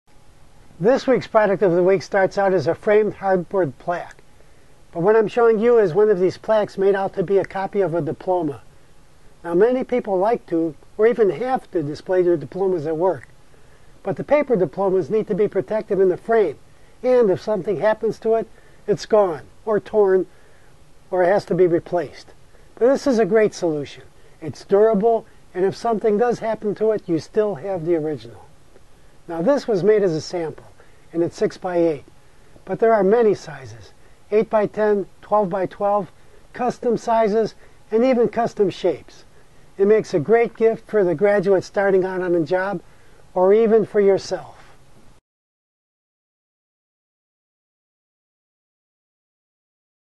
Terrific Originals Product of the Week 9-12-11
http (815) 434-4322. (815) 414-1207. The hardboard plaques featured this week are great for many things: copies of diplomas, full-colored awards, favorite photographs, and portraits. The image is sublimated onto the surface, and won't chip or scratch off.